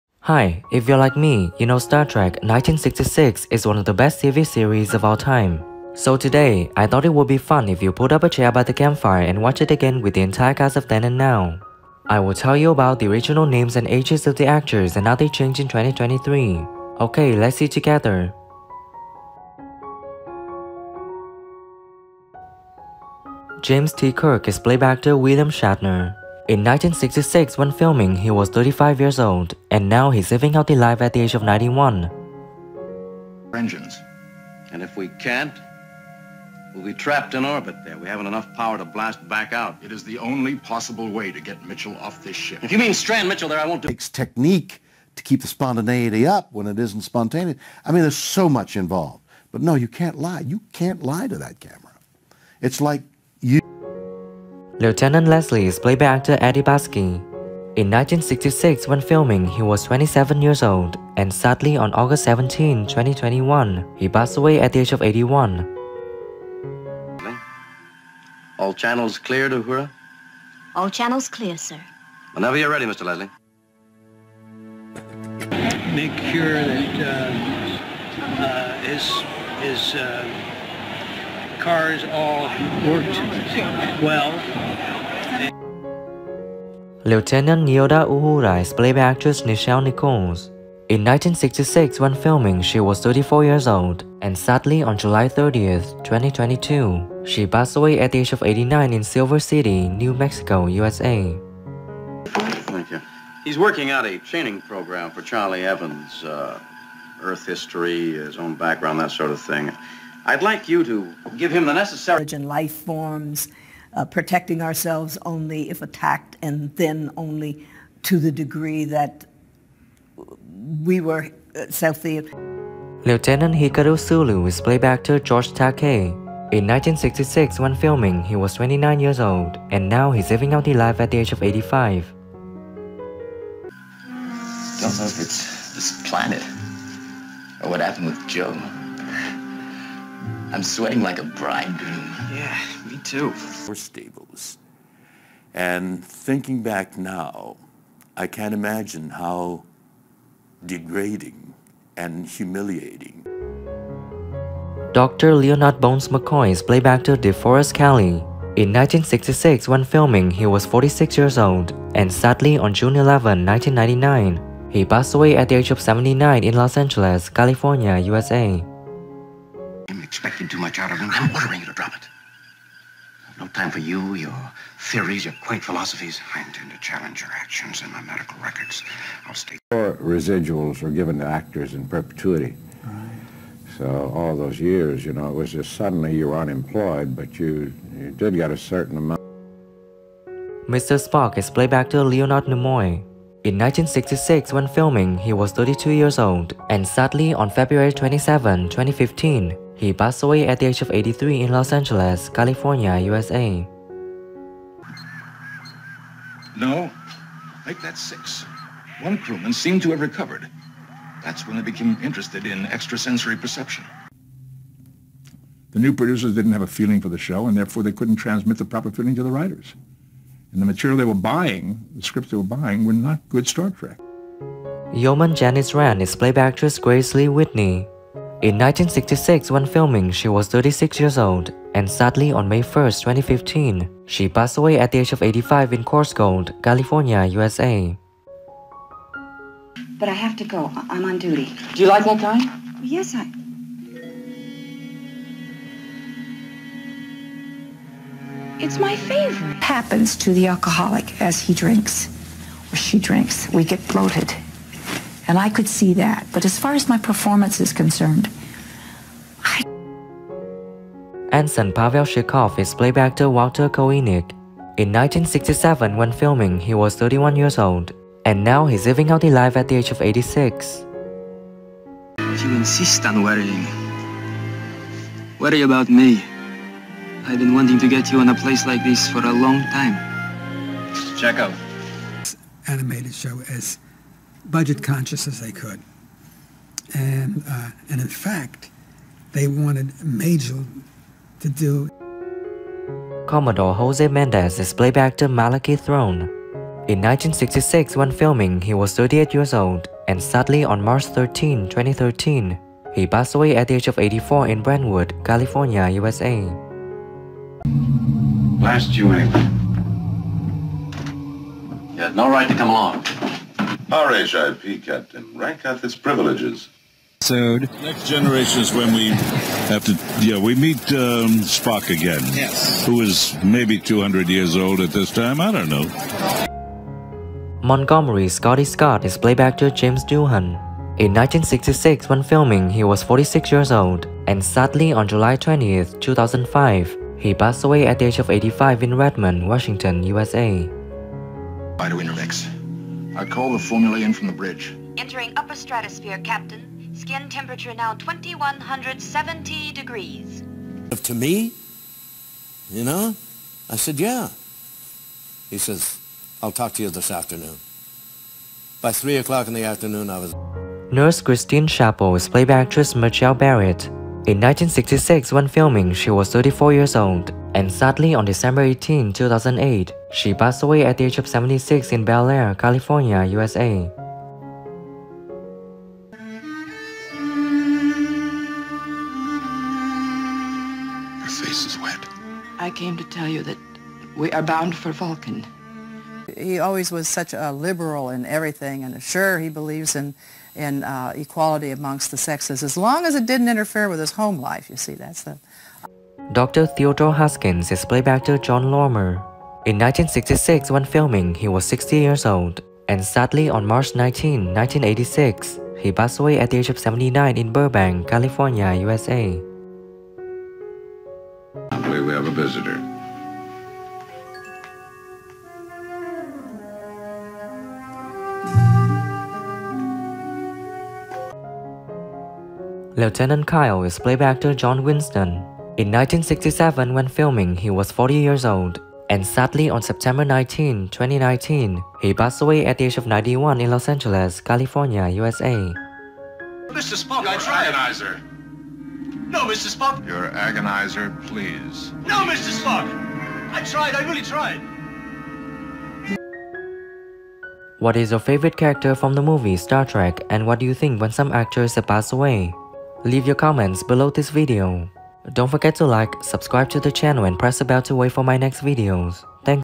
Star Trek: The Original Series (1966–1969) Cast: Then and Now 2021, All cast died tragically!
Star Trek is an American science fiction television series created by Gene Roddenberry that follows the adventures of the starship USS Enterprise (NCC-1701) and its crew. It later acquired the retronym of Star Trek: The Original Series (TOS) to distinguish the show within the media franchise that it began.

The show is set in the Milky Way galaxy, circa 2266–2269. The ship and crew are led by Captain James T. Kirk (William Shatner), First Officer and Science Officer Spock (Leonard Nimoy), and Chief Medical Officer Leonard H. "Bones" McCoy (DeForest Kelley). Shatner's voice-over introduction during each episode's opening credits stated the starship's purpose: